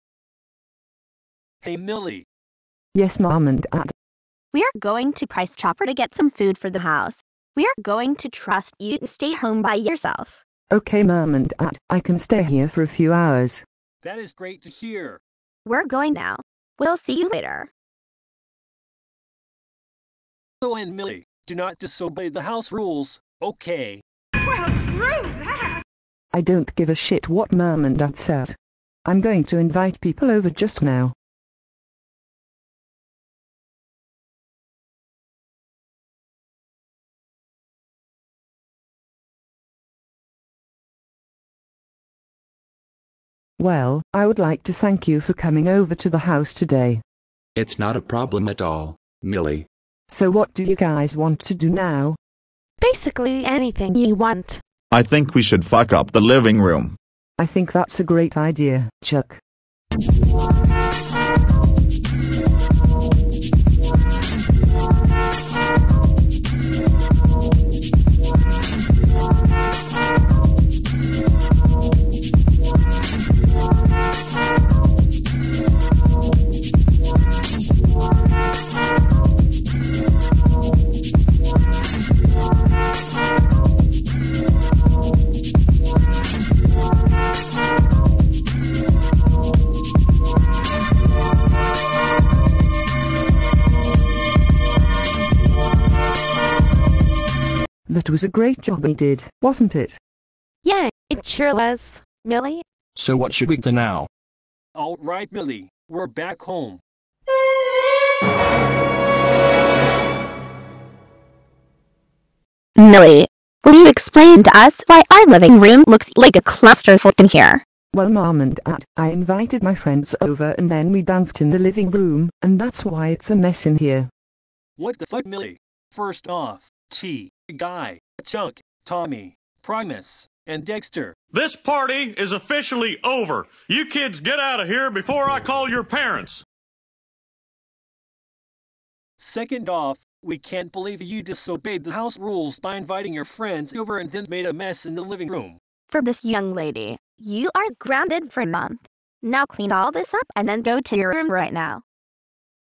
Millie Disobeys Her Parents and Gets Grounded
Millie invites her friends over even after her parents tell her not to and get grounded. Credit to everyone including RyGoesPop for Zypher and Trixy.
List of characters:
- Millie Kennedy
- Zypher Kennedy
- Trixy
- Tea
- Guy
- Chuck
- Tommy
- Promise
- Dexter